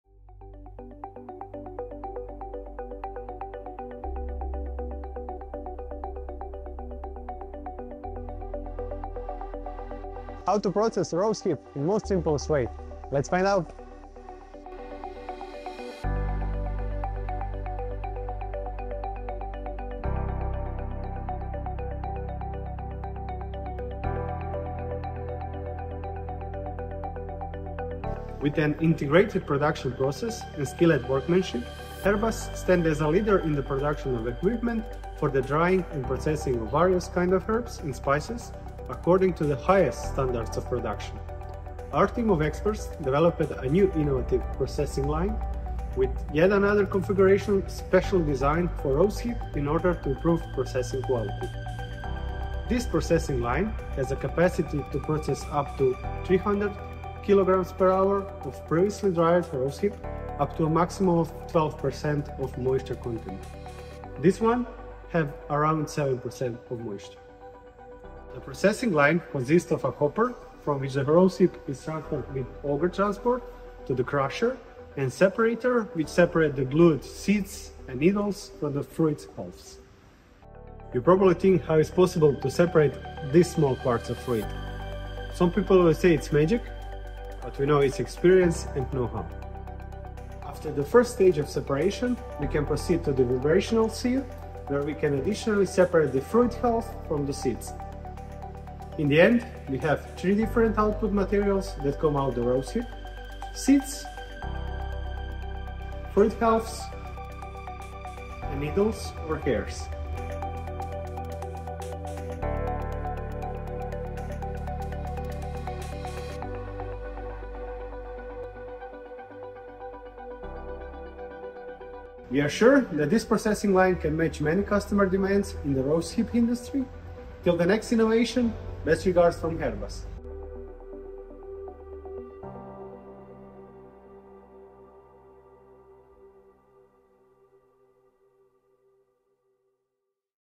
Processing line for rosehip
Rosehip, fruit of rose plant, is an interesting product. It consists of visually three distinct components: fleshy part with skin called the pericarp, seeds and iritating hairs that cause itching when they come in contact with human skin. Fleshy part of the fruit takes 80% of the total fruit mass and the rest are seeds and hairs. While fleshy parts are used to make tea and jam, seeds can be used to produce high-quality oil.

Fleshy parts of the fruit can be used to produce alcholic beverages, syrups or as food ingridients in different dishes.
Irritating hairs are especially problematic as they cause unpleasent skin irritation, so it is necessary to single them out (separate them from the rest of the fruit parts).

HERBAS line for processing rosehip finally separates rosehip into three main components: fleshy parts, seeds and irritating hairs. Process consists of following operations: equally dosing, opening and splitting of the fruit, pneumatic separation and mechanical separation. To be successful,  rosehip has to be dried to less than 12% of moisture content.

At the end of the line, there are clearly separated above mentioned components that are ready for the next processing steps.

Herbas d.o.o.
Kolodvorska 23a, Budančevica
48362 Kloštar Podravski, Croatia